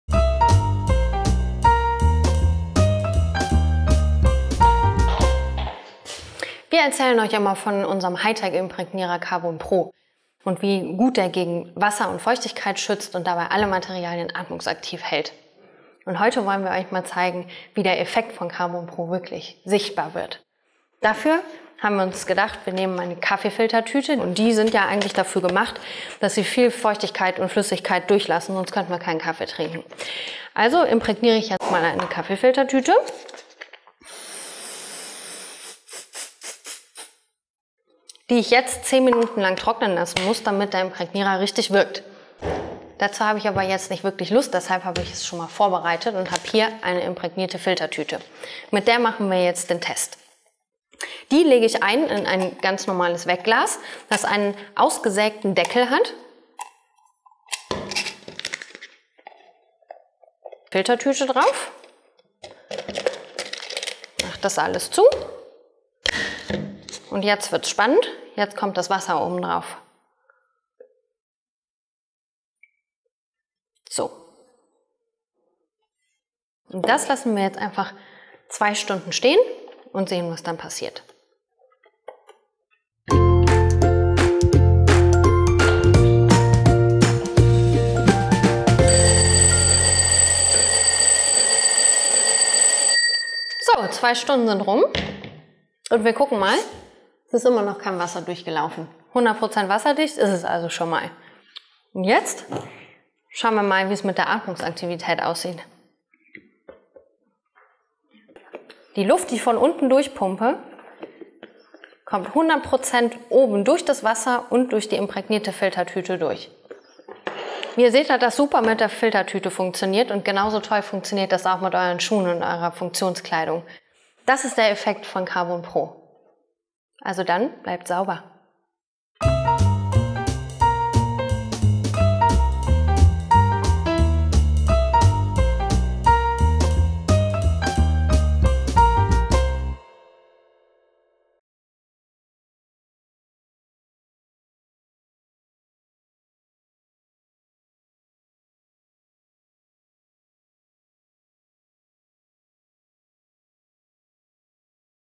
PutzFlash / Folge 3 - Das Filtertüten-Experiment
Carbon Pro ist unglaublich! Seine Eigenschaften -- 100% wasserdicht und atmungsaktiv -- sind unerreicht. In dieser Versuchsanordnung wird Carbon Pro mithilfe einer Filtertüte auf den Prüfstand gestellt.

Kaffeefilter sollen Kaffee filtern. Sprüht man sie aber mit Carbon Pro ein, ist es damit vorbei: Kein Tropfen gelangt mehr durch sie hindurch. Doch wie steht es mit der versprochenen Atmungsaktivität? Auch die kann in dieser Versuchsanordnung bewiesen werden. Seht selbst!